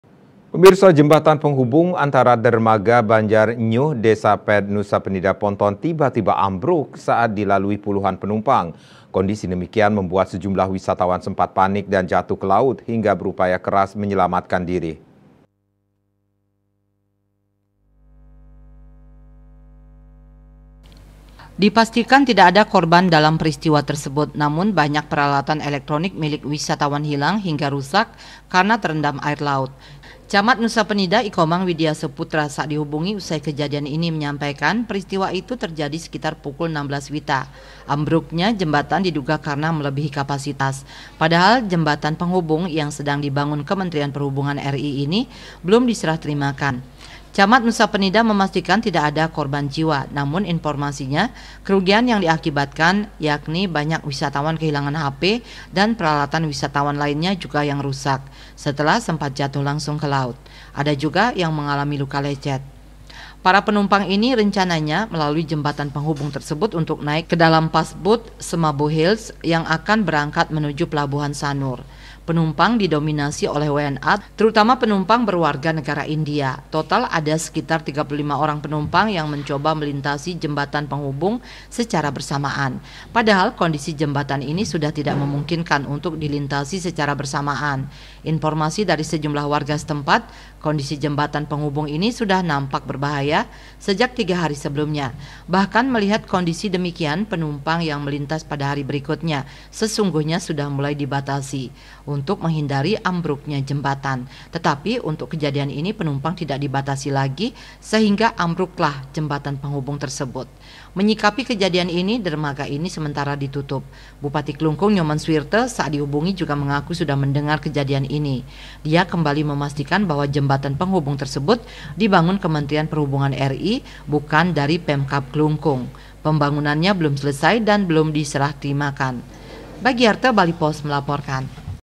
JEMBATAN PENGHUBUNG DERMAGA BANJAR NYUH NUSA PENIDA AMBRUK
JEMBATAN PENGHUBUNG ANTARA DERMAGA BANJAR NYUH DESA PED NUSA PENIDA PONTON, TIBA-TIBA AMBRUK SAAT DILALUI PULUHAN PENUMPANG. KONDISI DEMIKIAN MEMBUAT SEJUMLAH WISATAWAN SEMPAT PANIK DAN JATUH KE LAUT, HINGGA BERUPAYA KERAS MENYELAMATKAN DIRI.